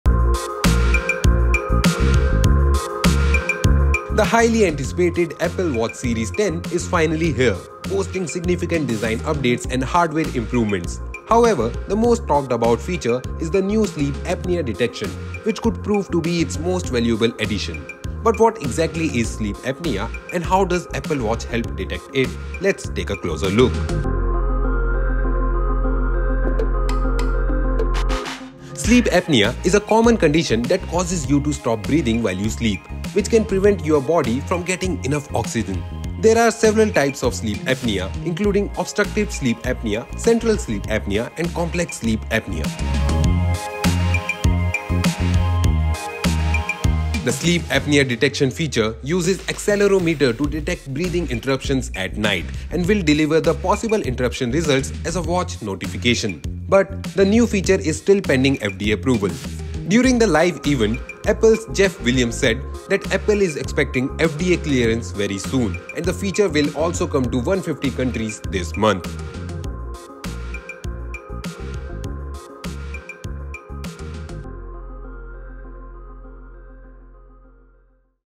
Apple New Innovation: Sleep Apnea -What It Is And How The Apple Watch Can Help Detect It? iPhone 16
Sleep Apnea: What It Is And How The Apple Watch Can Help Detect It?

The highly anticipated Apple Watch Series 10 is finally here, boasting significant design updates and hardware improvements. However, the most talked-about feature is the new sleep apnea detection, which could prove to be its most valuable addition. But what exactly is sleep apnea, and how does the Apple Watch help detect it? Let’s take a closer look.
What is Sleep Apnea?
Sleep apnea is a common condition that causes you to stop breathing while you sleep, which can prevent your body from getting enough oxygen. There are several types of sleep apnea, including obstructive sleep apnea (OSA), central sleep apnea, and complex sleep apnea
How Does Apple Detect Sleep Apnea?
The sleep apnea detection feature uses the accelerometer to detect breathing interruptions at night, and will deliver the possible interruption results as watch notifications. But the new feature is still pending FDA approval, Apple's Jeff Williams said during the livestreamed event that. Apple's expecting FDA clearance "very soon," and the feature will also come to 150 countries this month.